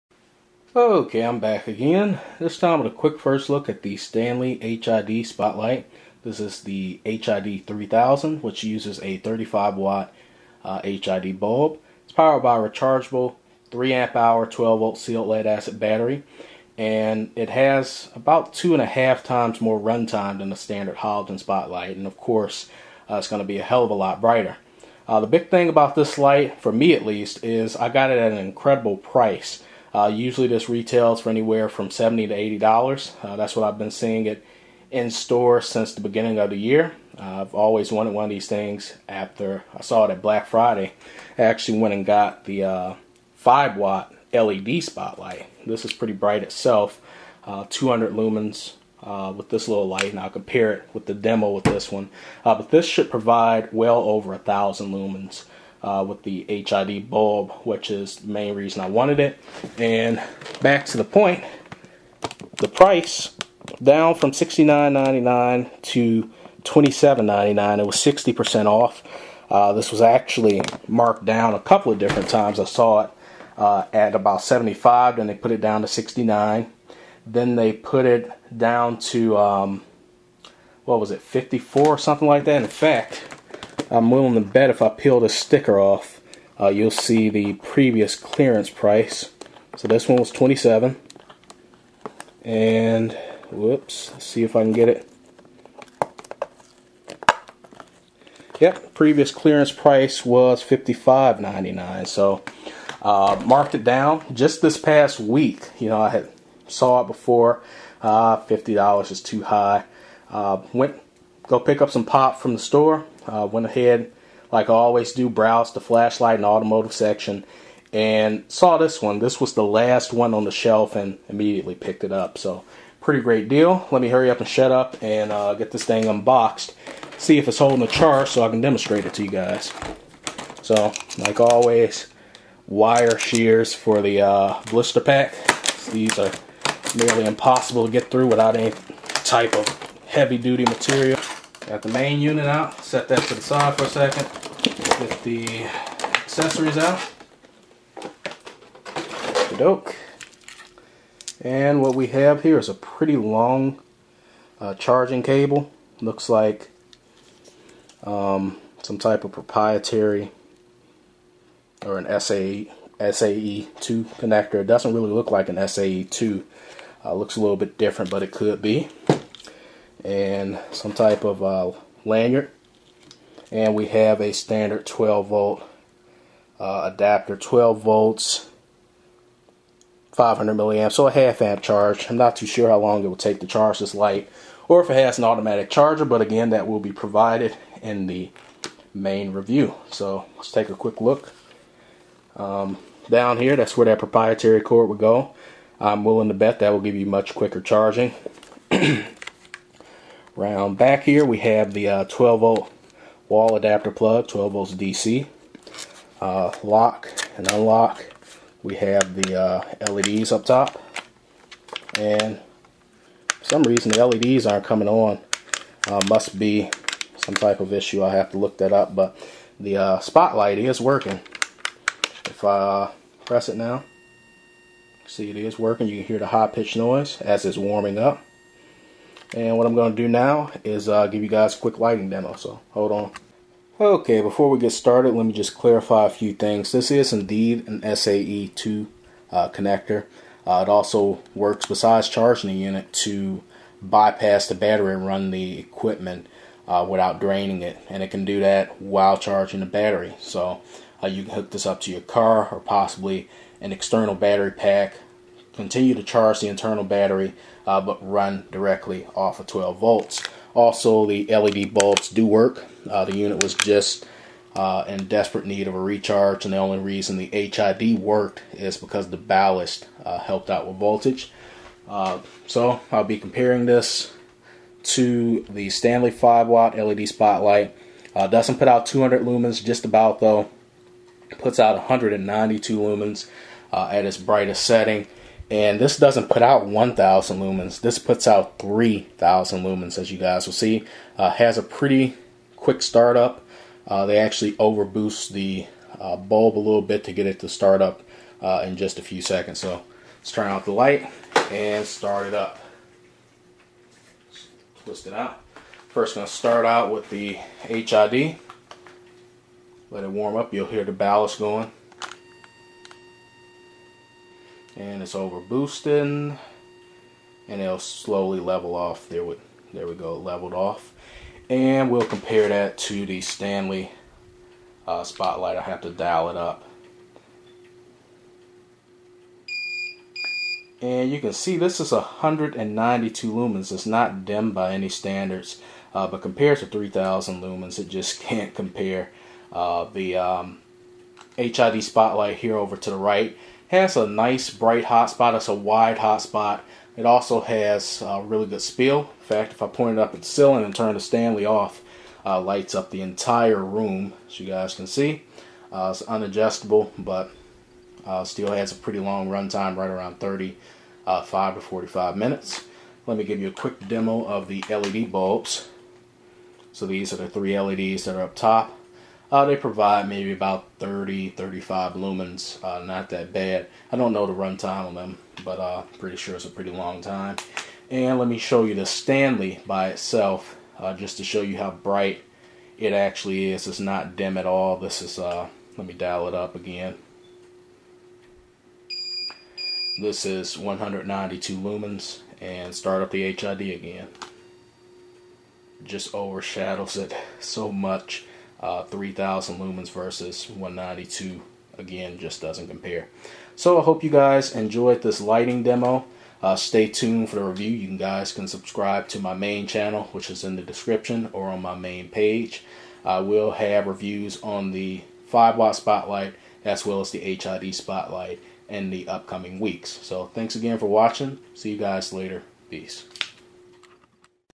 Stanley HID Spotlight 3000 Unboxing and Lighting Demo
Unboxing as well as light comparison between this 3000 lumen HID beast and the stanley 5 watt led spotlight, producing 192 lumens. I got a great deal on the hid spotlight, only costing $30. It components include a 12 volt 3 ah battery, 35 watt hid bulb, 3 led area light, carry lanyard, wall charger as well as a 12 volt charger / external power connector.

product on amazon -